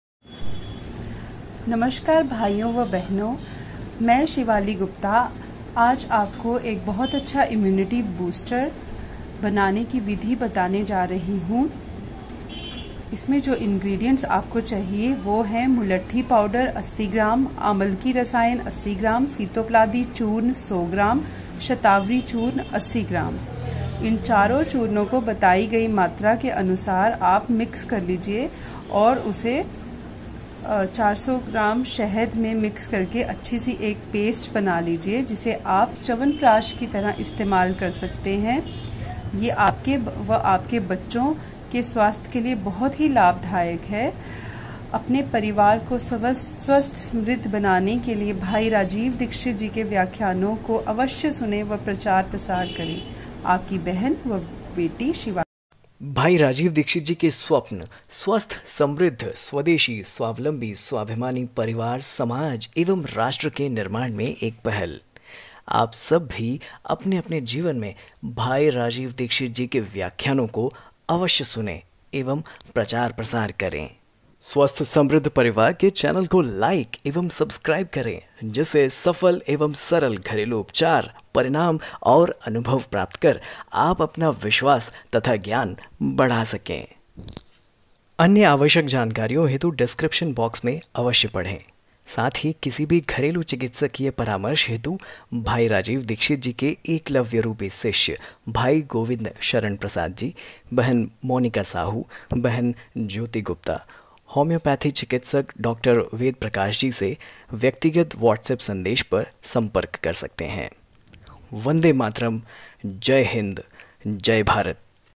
क्यों नहीं पड़ते बीमार स्वस्थसमृद्धपरिवार के सदस्य घर मे बनाये शुद्ध इम्युनिटी बूस्टर
घर मे बनाये शुद्ध इम्युनिटी बूस्टर
मुलेठी,आमलकी रसायन,शतावरी 80 80 ग्राम,
सीतोपलादी 100 ग्राम शहद 400 ग्राम मिलाकर रखें व एक से दो चम्मच सेवन करें गुनगुने दूध या पानी के साथ वन्देमातरम जयहिंद